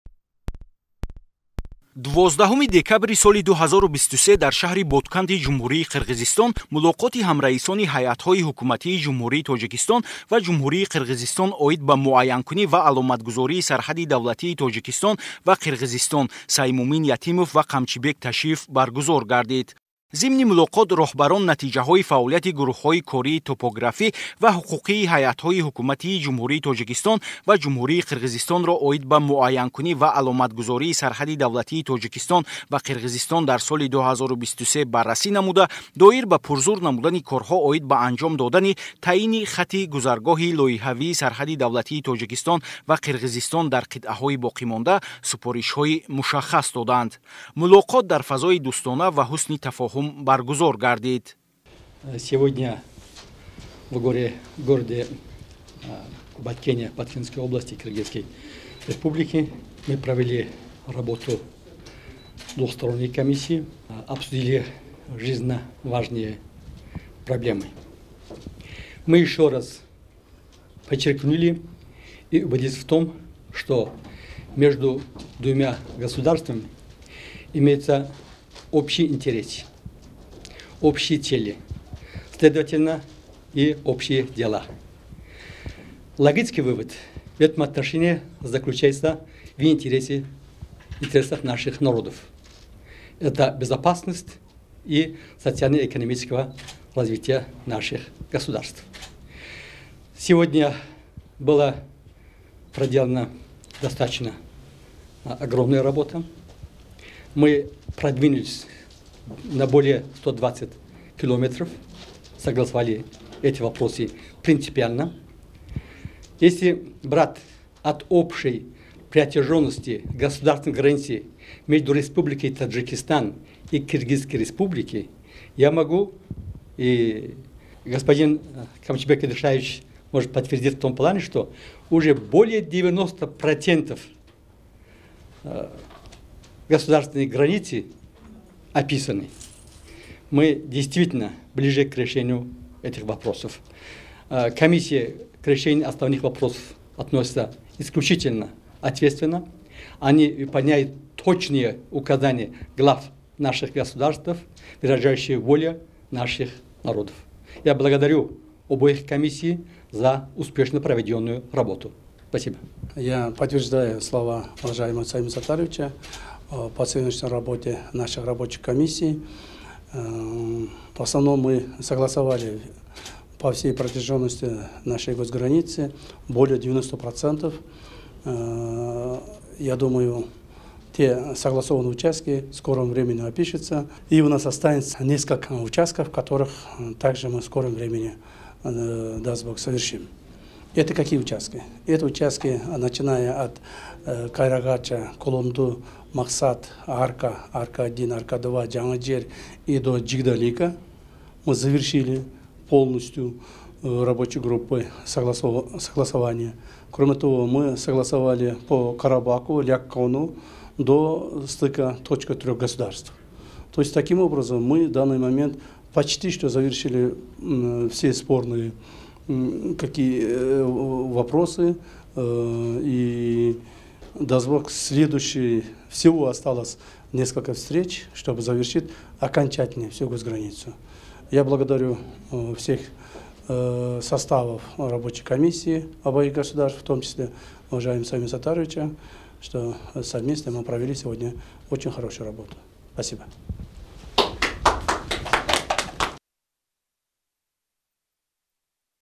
Состоялась встреча сопредседателей Правительственных делегаций Республики Таджикистан и Кыргызстан
12 декабря 2023 года в  городе Баткене Кыргызской Республики состоялась встреча сопредседателей Правительственных делегаций Республики Таджикистан и Кыргызской Республики по делимитации и демаркации таджикско-кыргызской государственной границы Саймумина Ятимова и Камчыбека Ташиева. Об этом НИАТ «Ховар» сообщили в Государственном комитете национальной безопасности Республики Таджикистан.

В ходе встречи руководители, обсудив итоги проделанной топографическими и правовыми рабочими группами Правительственных делегаций Республики Таджикистан и Кыргызской Республики по делимитации и демаркации  таджикско-кыргызской государственной границы работы в 2023 году, дали конкретные поручения по активизации работы по завершению описания прохождения проектной линии таджикско-кыргызской государственной границы на оставшихся участках.

Встреча прошла в атмосфере дружбы и взаимопонимания.